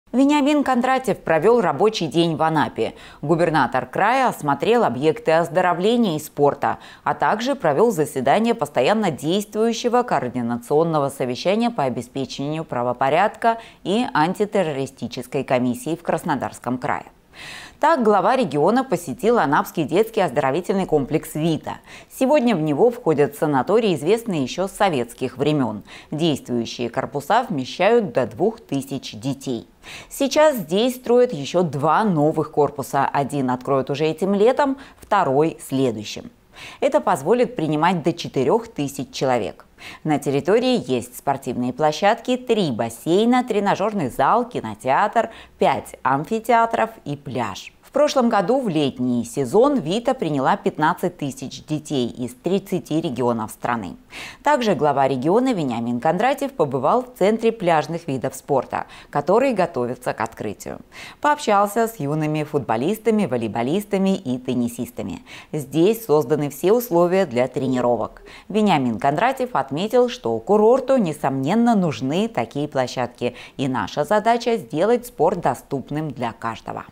Губернатор Кубани посетил детский комплекс «Вита» и Центр пляжных видов спорта в Анапе
Сегодня Вениамин Кондратьев находится на курорте с рабочим визитом. Вместе с главой региона объекты оздоровления и спорта осмотрели руководители правоохранительных структур Краснодарского края и мэр Анапы Василий Швец.

Действующие корпуса оздоровительного комплекса «Вита» вмещают до 2 тысяч детей. Гендиректор Василий Димоев сообщил, что сейчас строят два новых корпуса, один откроется этим летом, второй – следующим. Это позволит принимать до 4 тысяч человек. На территории есть спортплощадки, бассейны и амфитеатры, тренажёрный зал, кинотеатр. Собственный пляж оснащен теневыми навесами, медицинскими и спасательными постами. В летний сезон-2023 «Вита» приняла 15 тысяч детей из 30 регионов страны.

– Анапа всегда была и должна остаться главной детской здравницей России. Детские санатории – это не просто бизнес, прежде всего – социальная ответственность. Важно, чтобы ребята уехали только с положительными эмоциями, чтобы воспоминания об отдыхе остались с ними на всю жизнь, – сказал Вениамин Кондратьев.

Также Губернатор посетил Центр пляжных видов спорта. В новом спортобъекте есть поля для соревнований и тренировок, трибуны более чем на 1200 мест. Система видеонаблюдения подключена к аппаратно-программному комплексу «Безопасный город».

– В Анапе очень востребованы пляжные вида спорта. Ежегодно мы встречаем спортсменов со всей страны на крупные соревнования по пляжному футболу, гандболу, волейболу и теннису, проводим финалы Кубка и Чемпионата России, –
рассказал Губернатору Василий Швец.
 – В частности, скоро стартует этап Чемпионата России по пляжному футболу, его площадкой станет этот новый объект